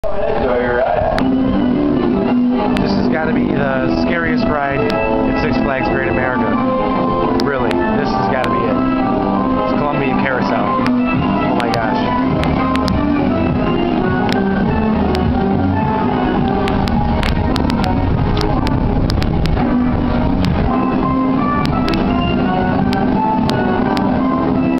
Columbia Carousel On Ride at Six Flags Great America
This is the grandest of the grand, Columbia Carousel, at Six Flags Great America. This double-decker carousel, or for kiddies, merry go round, is the landmark upon entering the park. Isn't this the scariest ride ever created by Six Flags? Ha ha!!